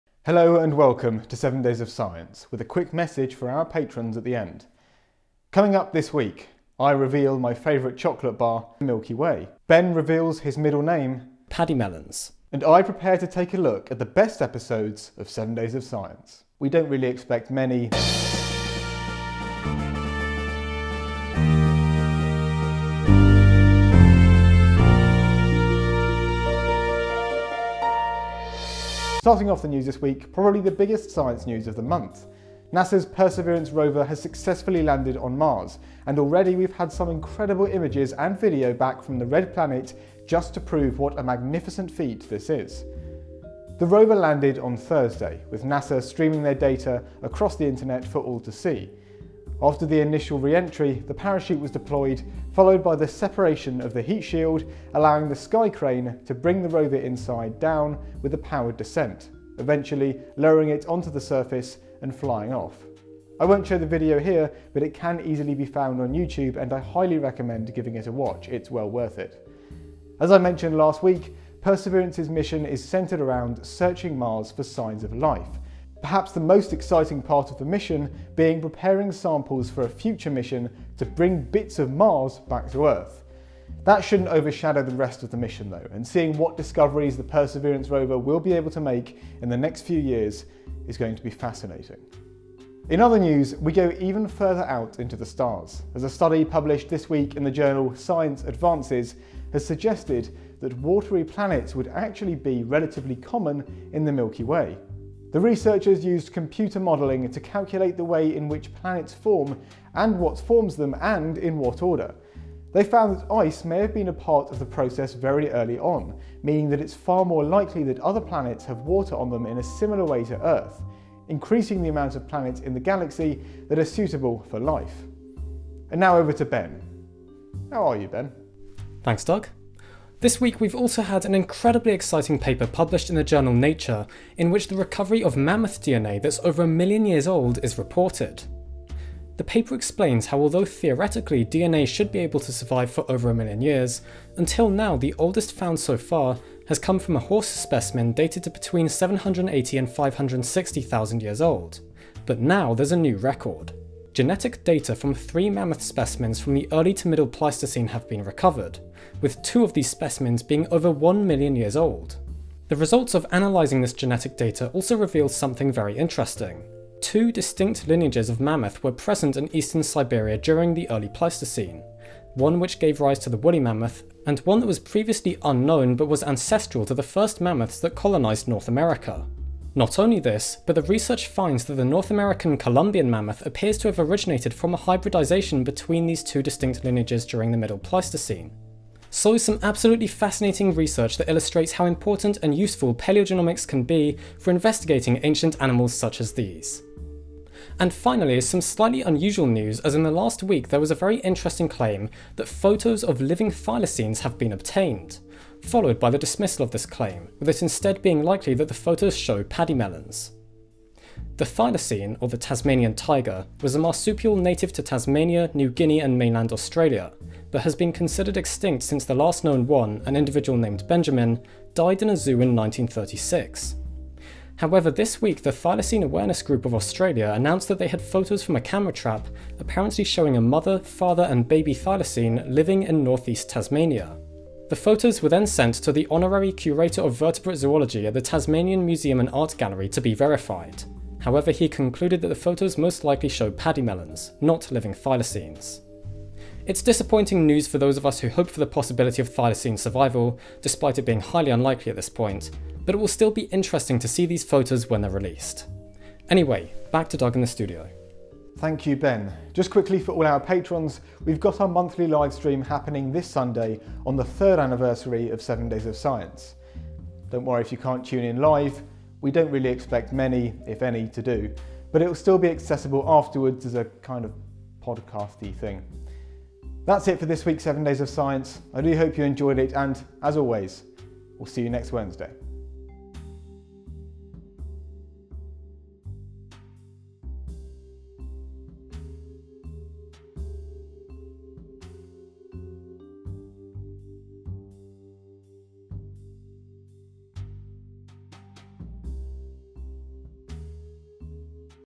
Million-Year-Old Mammoth DNA Discovered | 7 Days of Science
In this week's episode of 7 Days of Science, NASA's Perseverance rover touches down on Mars (and sends us awesome pictures), water planets are more common than we thought, and some very old DNA is discovered...